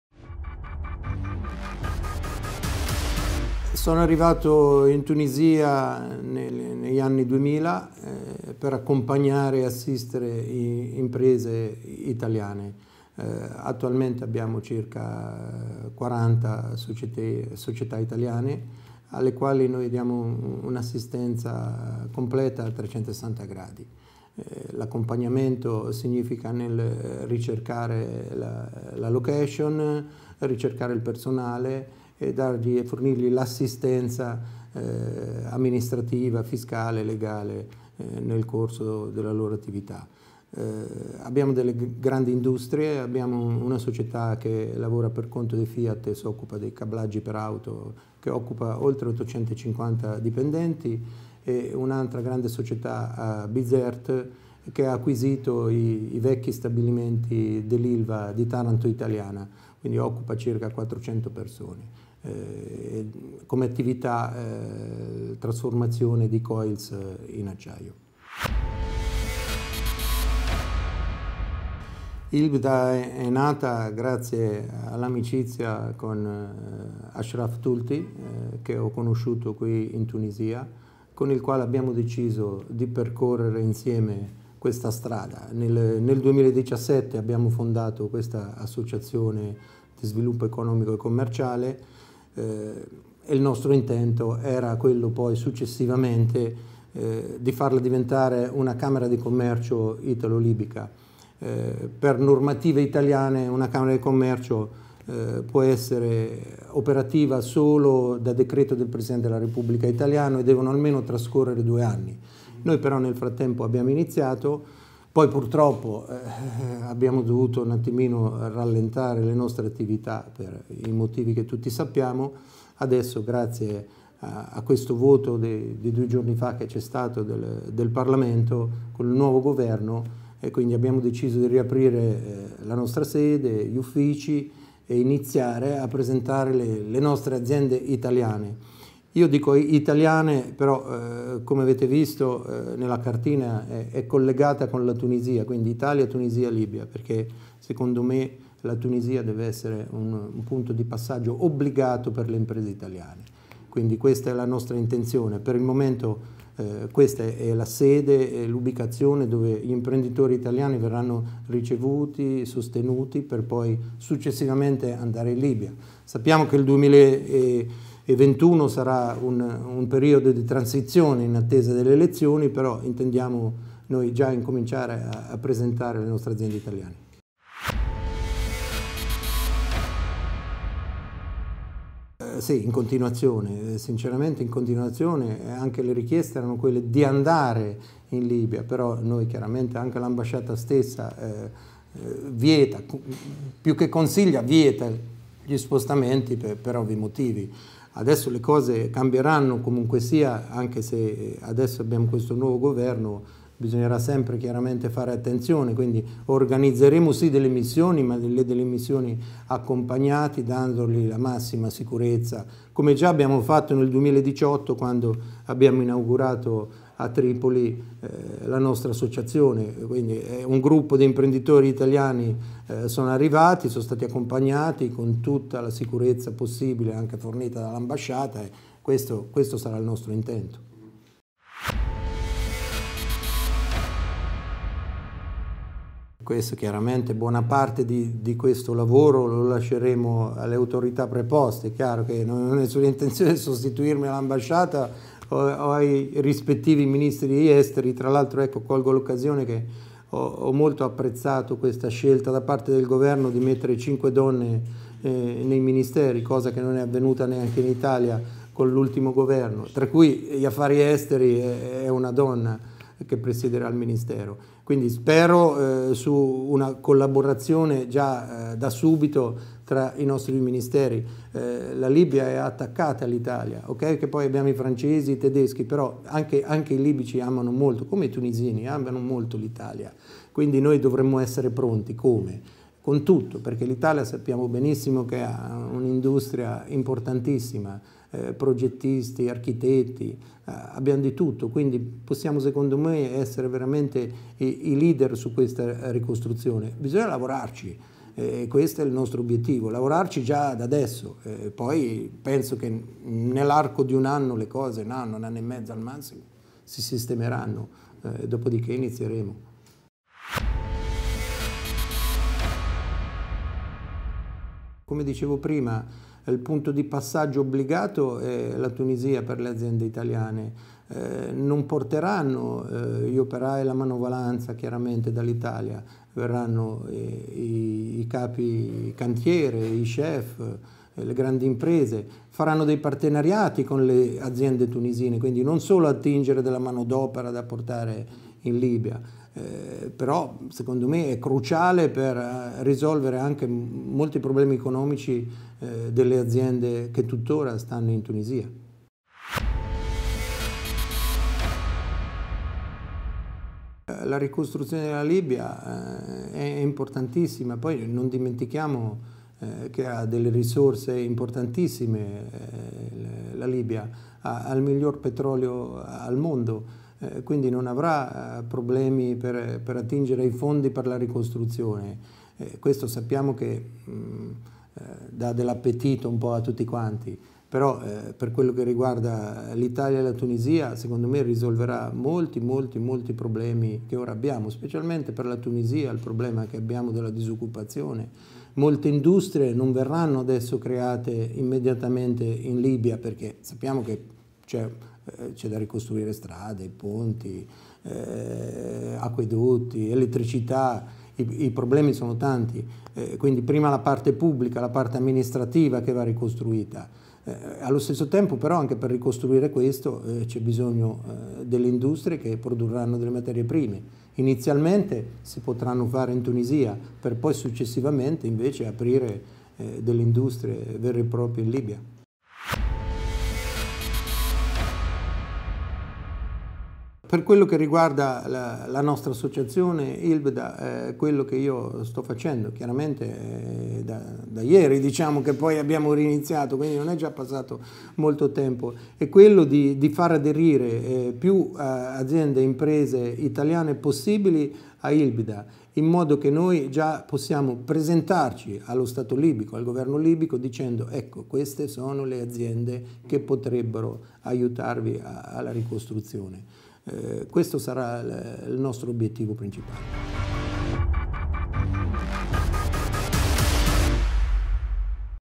Sandro Fratini : la Tunisie est un passage obligatoire pour les entreprises italiennes
Sandro Fratini président d'ILBDA : la Tunisie est un passage obligatoire pour les entreprises italiennes avant de participer à la reconstruction de la Libye